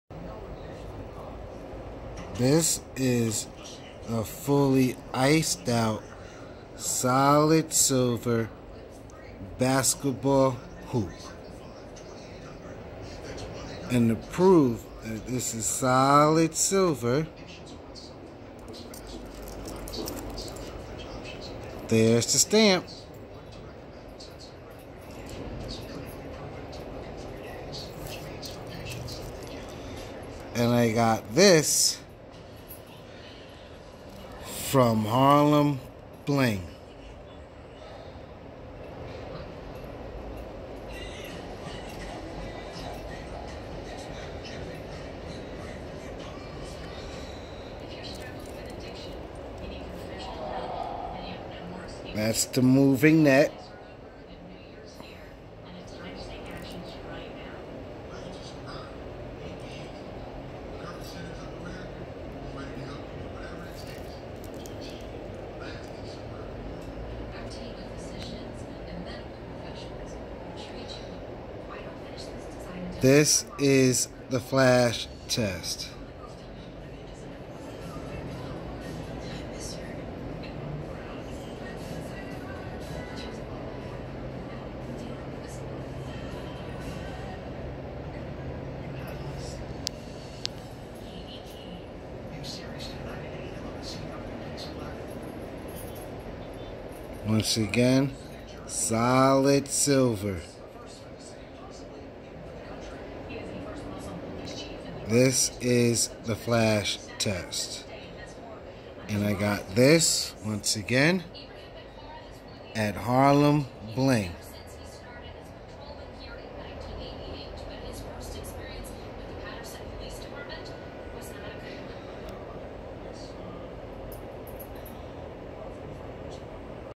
Harlembling fully iced out basketball🏀hoop w/moving net review flash test🌟
I hope everyone enjoy this video. This is the flash test using artificial light🌟 to show how the fully iced out basketball🏀hoop diamond's💎dance's💃. I got this because I do play ball but mostly becuase to pay tribute to KOBE BRIAN and his baby girl G.G. That being said I do not sponsor them I just really like their merchandise. If anyone is interested in getting any of Harlem bling merchandice their website is Harlembling.com. They have EXQUISITE QUALITY.